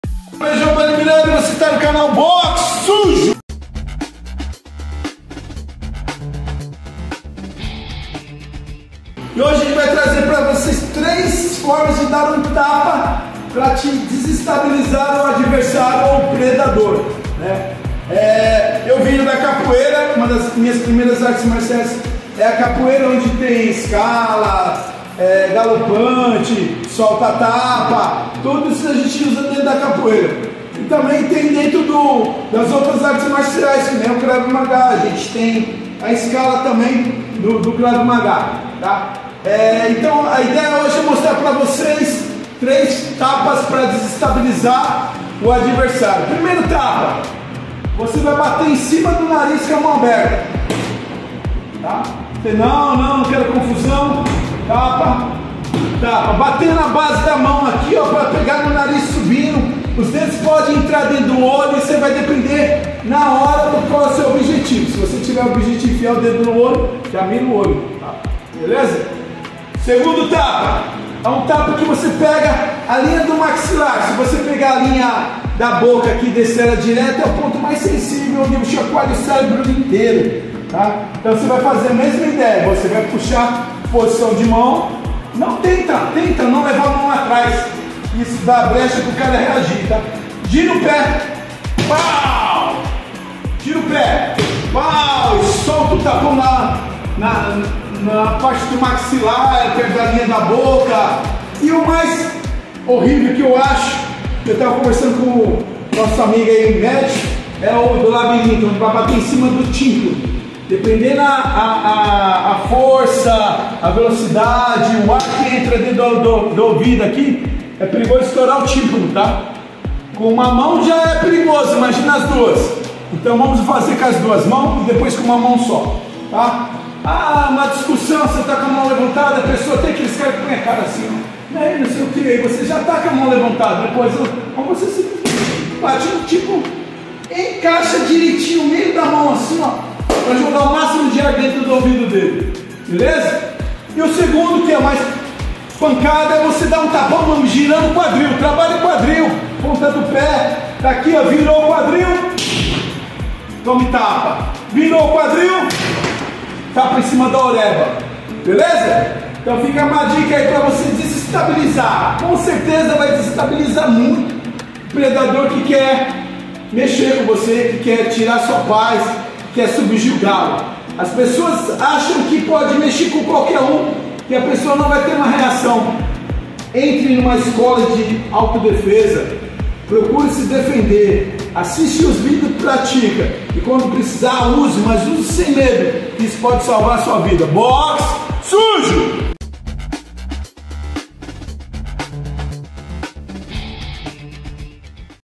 BOXE SUJO | 3 TAPAS PARA DESESTABILIZAR O ADVERSÁRIO | APRENDA EM CASA
Neste vídeo da série "Aprenda em Casa", o instrutor Giovani Miranda te ensina 3 tapas para desestabilizar o adversário.

🥊BORA TREINAR?!